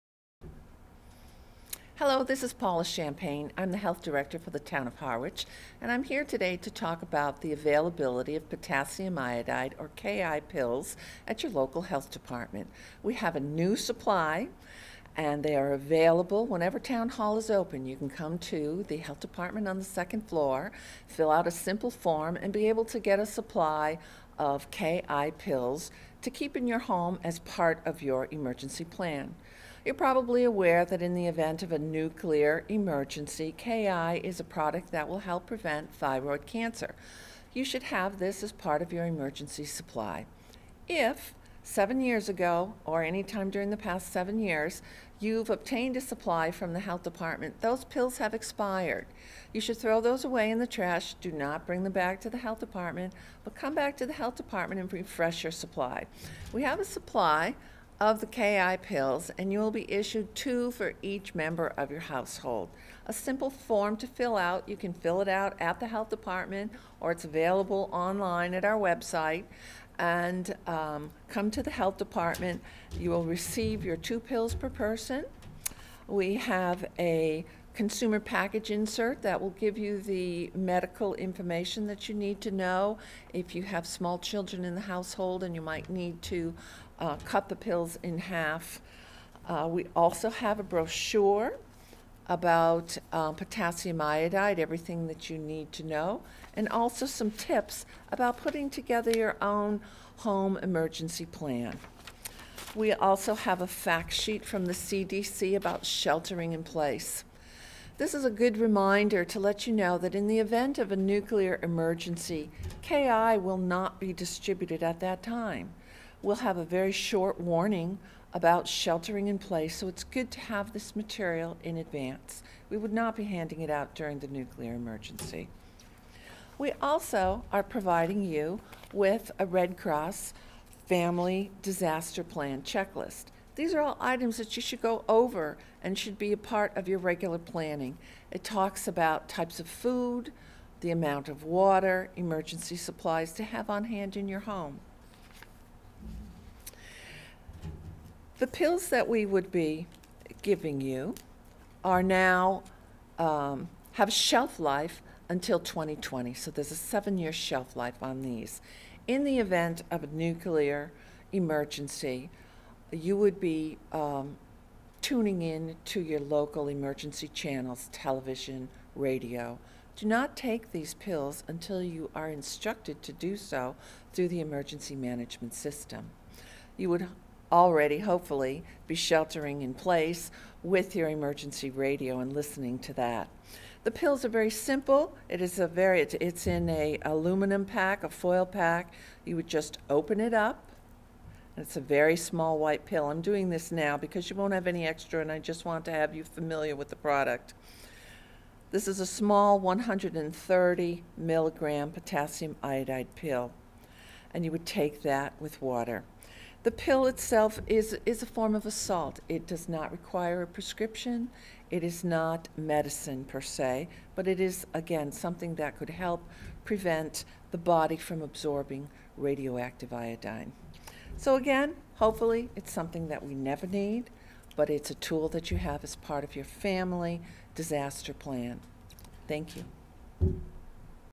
Harwich Health Director Talks About Ki pill distribution 2013
Harwich Health Director Paula Champagne discusses the availability of Ki (potassium Iodide) pills in case of a nuclear emergency.